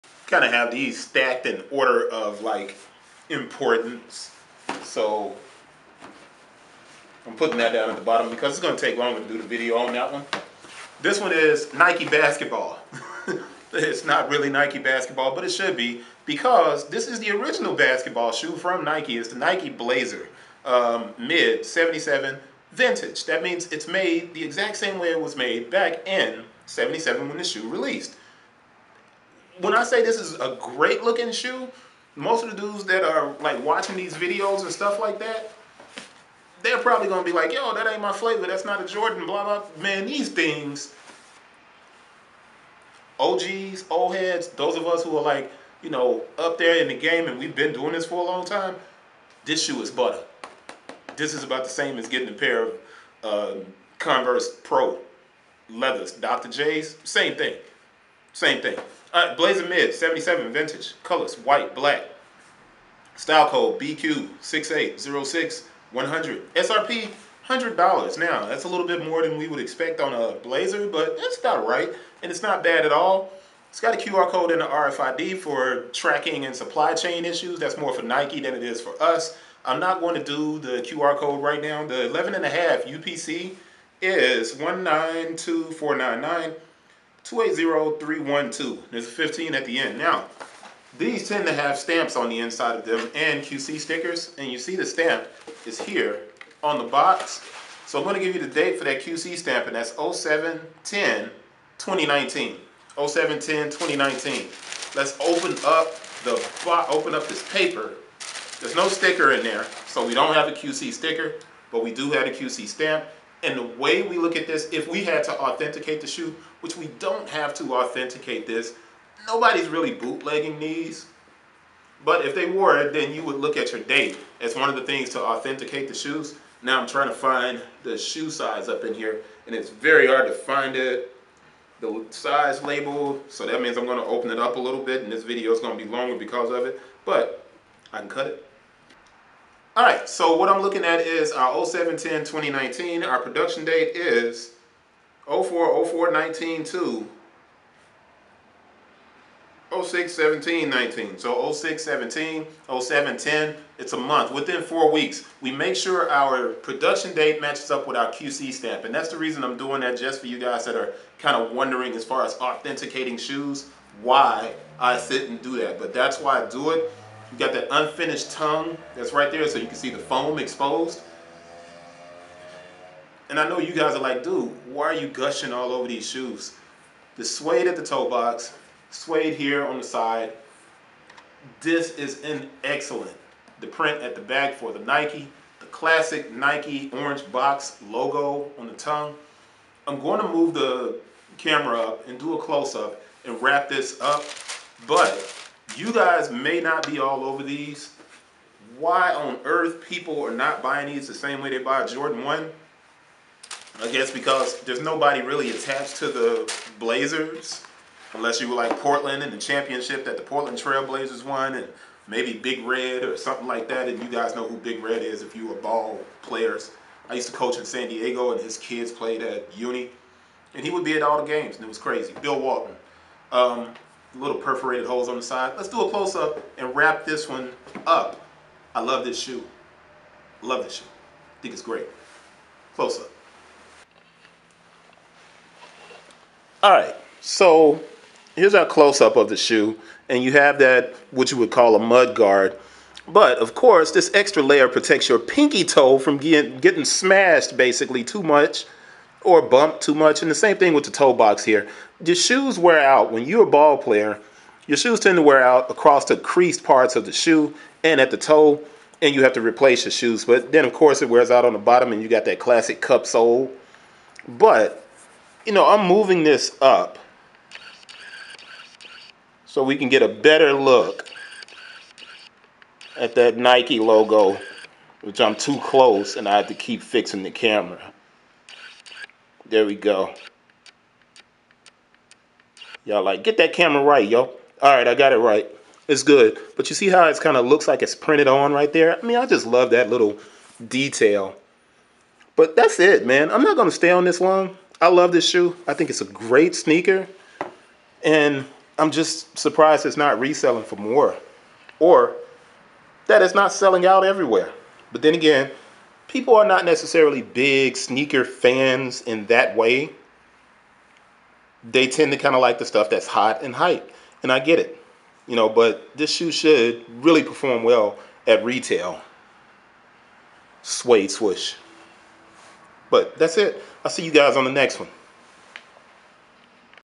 Nike Blazer Mid '77 Vintage White/Black BQ6806-100
Learn more about the sneaker industry by picking up my book: "Nike's Consumer Direct Offense, Amazon and StockX: The Disruption of Sneaker Retail"

The original basketball shoe from Nike arrives in a vintage release that looks like the original version including the light orange Nike logo box. The shoe features suede at the toebox and on the lateral sidewall to give the shoe more structure and stability. The NIKE print at the heel definitely feels vintage and while many don't think highly of the Blazer and it's been adopted by skate culture, the shoe remains a timeless classic that is simple in design and pairs well with a number of clothing options casually.